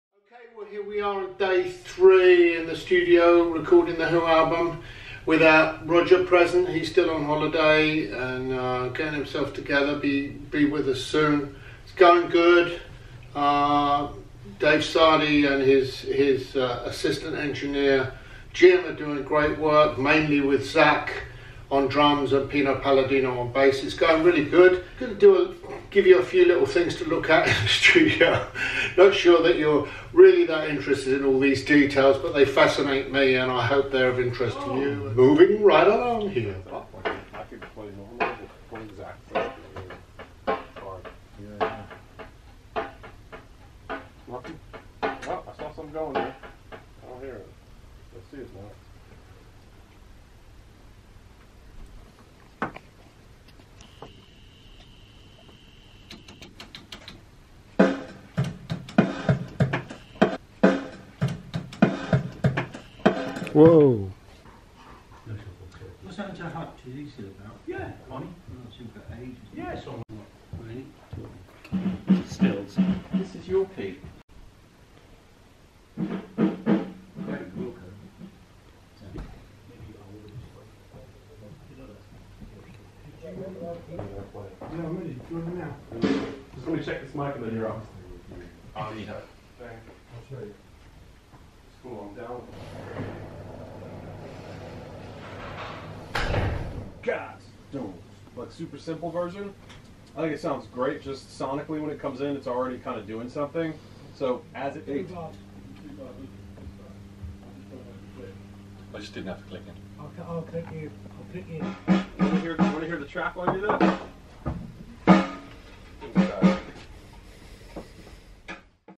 Pete Townshend's Vlog: Day 3 ~ The Who Studio Sessions
4 February 2019. Day 3 of recording the new Who album. Today we take a look at some of the equipment we're using here at British Grove Studios. the official web site for The Who, Pete Townshend and Roger Daltrey.